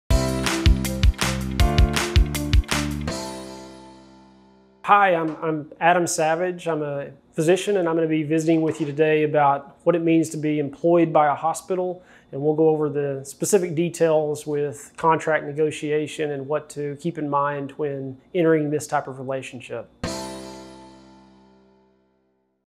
Hospital employment, RVUs and contract negotiation teaser
This is just a short video on what Dr. Adam Savage will discuss in his video on hospital employment as a physician.

How do doctors get paid by RVUs? How much do the doctors get paid || Business Savvy Physician

Watch Dr. Adam Savage explain what an RVU is and how a hospital pays you. This is just a segment from the full video on "Contract Negotiations and Understanding RVUs."

✅ Please watch this videos also if you are interest 🙏🙏🙏

Cheers to growing your practice,
Business Savvy Physician